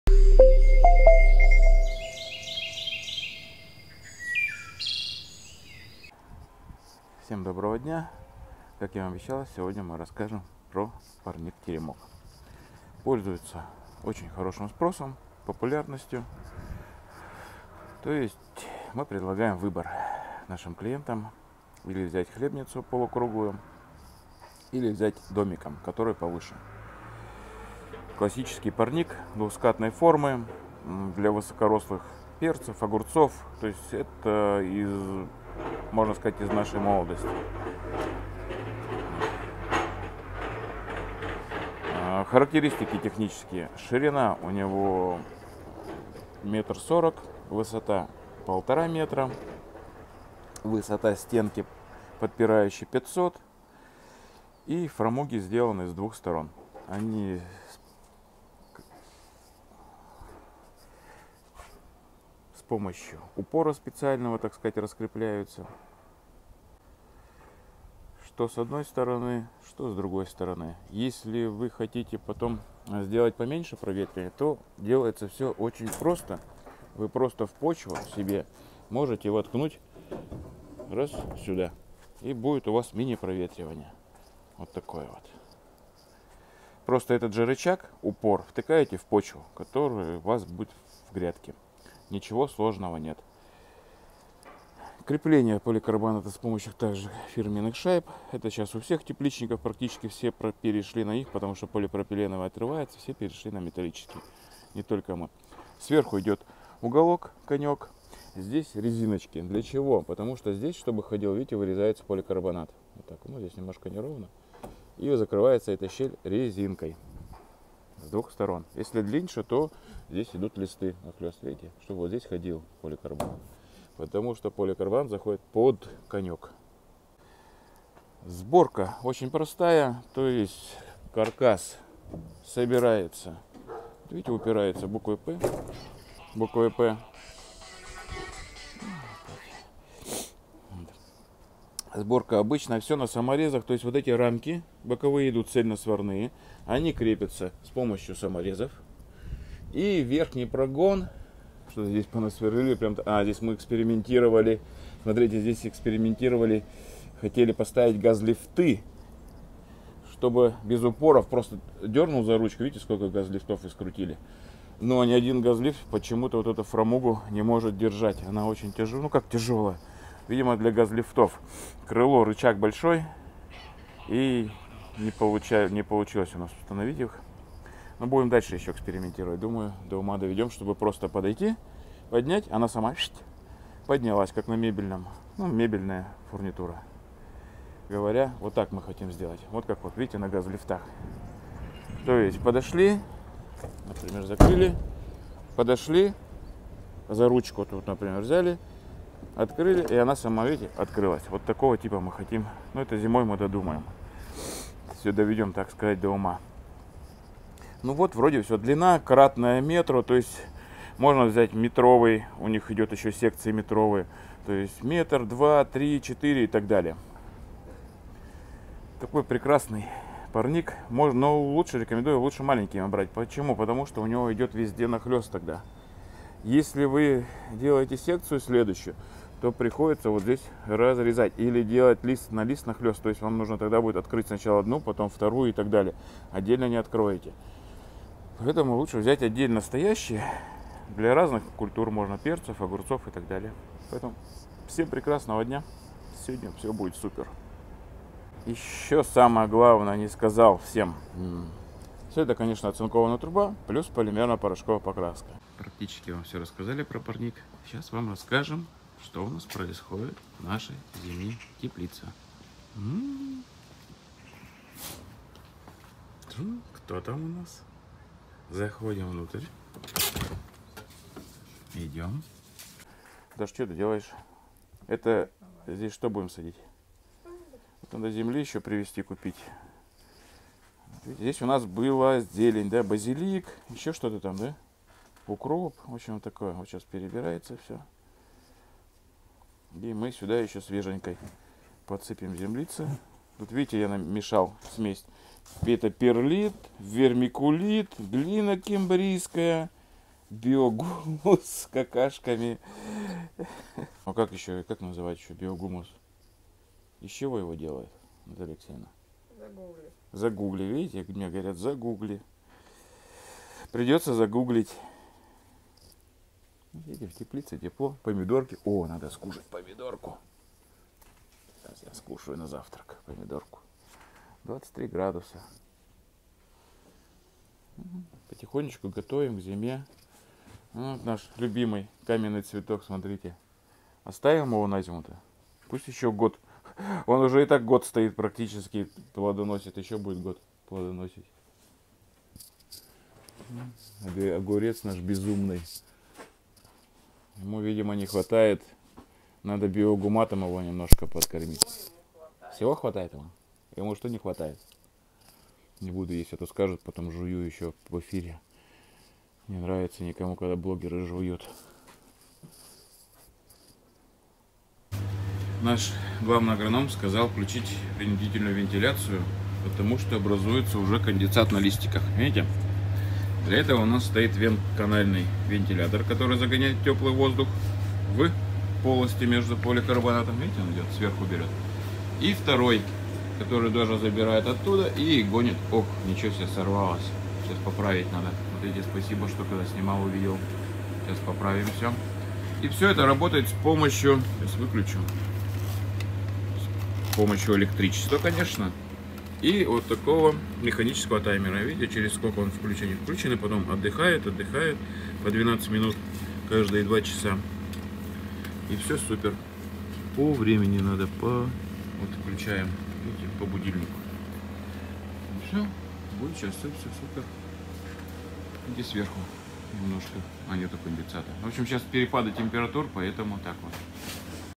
ПАРНИК "ТЕРЕМОК" ДЛЯ ПЕРЦЕВ И ОГУРЦОВ!
Парник «Теремок»

Бывает, что нужно посадить отдельно разные культуры? Совсем негде поставить теплицу на участке? Или нужен вариант подешевле?
Тогда обращаем ваше внимание на парник «Теремок». Это полноценная качественная конструкция, особой формы и размера. Форма в виде домика обеспечивает большой внутренний объём. С двух сторон открываются большие фрамуги для полноценного проветривания. Чтобы можно было полностью открыть и зафиксировать, стоят специальные упоры. С двух сторон фрамуг есть цепи для предотвращения опрокидывания при сильном ветре.
Каркас парника, также как и теплицы, сделан из оцинкованной трубы с порошково-полимерным покрытием. Устанавливается на фундамент. Сверху парник покрыт качественным сотовым поликарбонатом. Вся фурнитура в комплекте.
Ширина парники 1,4 м, высота 1,5 м, длина от 2 м.
За расчетом и консультацией
🏡 Екатеринбург ул Климовская 23
🖥 ural-usadba.ru